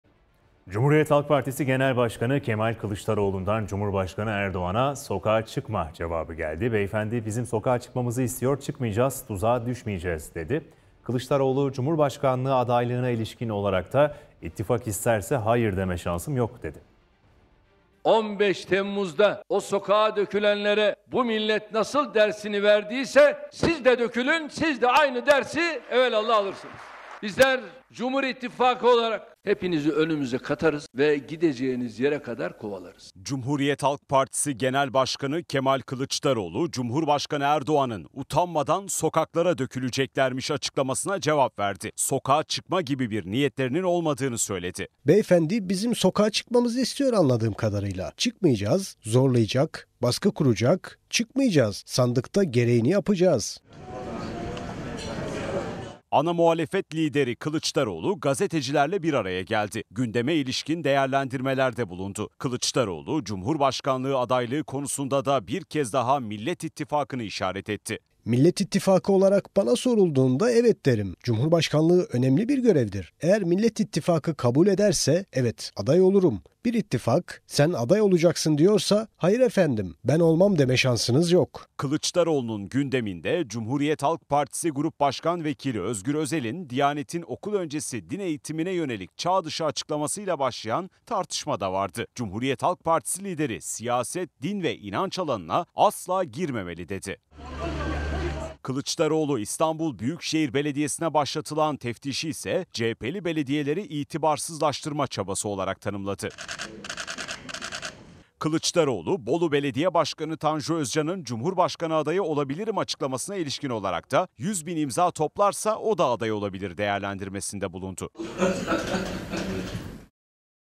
CHP'de Kılıçlar Çekildi: '100 Bin İmza Toplarsa Aday Olabilir' - TGRT Haber
Basın mensupları ile bir araya gelen CHP Genel Başkanı Kemal Kılıçdaroğlu, Gündemi değerlendirdi. CHP lideri Tanju Özcan'ın açıklamaları ile ilgili: "Herkesin aday olduğu bir ortamda Tanju Bey de 'ben de aday olabilirim' diyor. 100 bin imza toplarsa o da aday olabilir" ifadelerini kullandı.